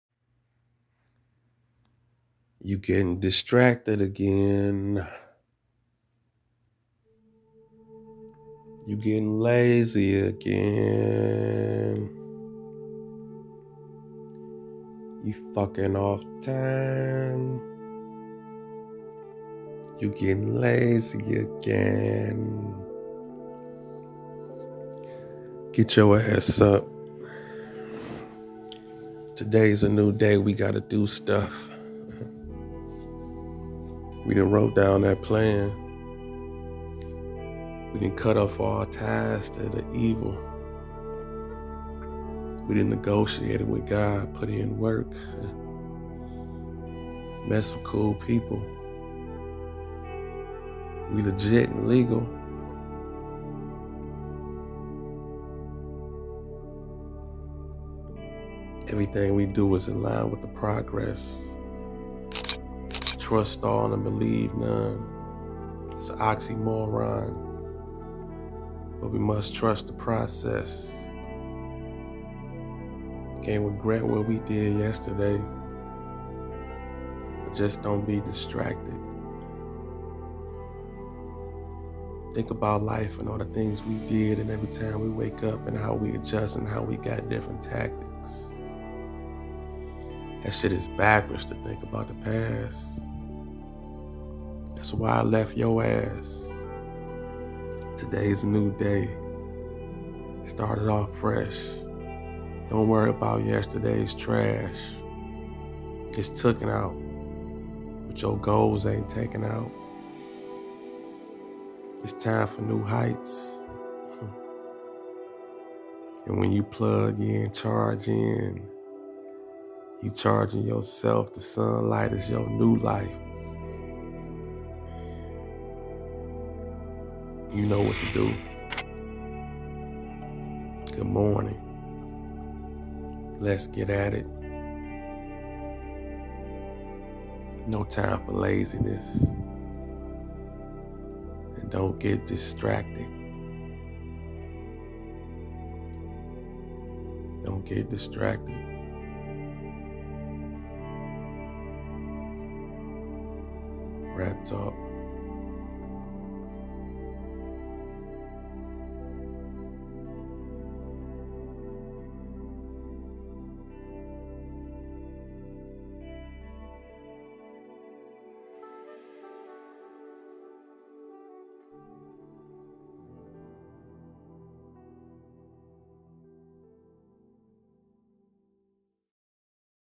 Stop Your Laziness, Don't Get Distracted - Raptalk Poetry Motivation To Inspire
Stop Your Laziness. Stop Getting Distracted Poem Inspired by @AbsoluteMotivation Motivational Speech Video